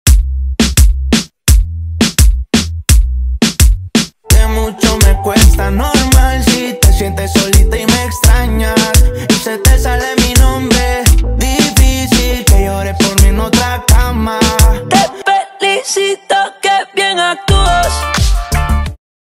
Febrero Total en Club 06: ¡Fiestas Todo el Mes en Escapade Houston!
Febrero se ilumina con el mejor ambiente en Club 06, dentro de Escapade Houston, donde la fiesta nunca termina. Prepárate para una experiencia sin igual, con una selección de reggaetón, hip hop y más, asegurando la música perfecta para cada noche. En Club 06, cada momento promete diversión, ritmo y puro baile. Deja atrás el frío y enciende tus noches con nosotros, donde cada beat celebra la pasión por la música urbana.